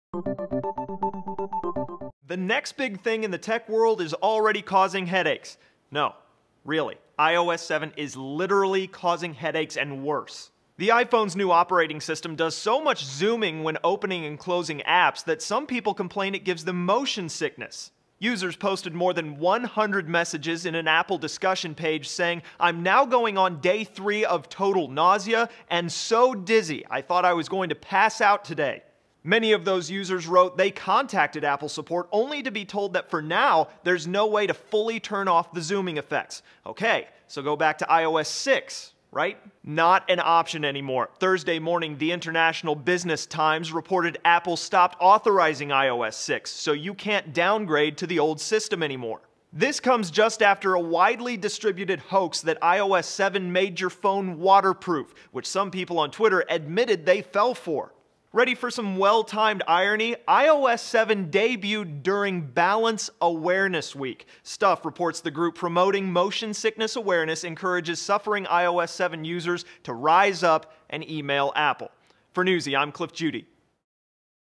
iOS 7 Motion Sickness Sufferers: For Now, Buck Up
Some say the zooming effects of opening and closing apps makes them sick, but there's currently no way to disable them.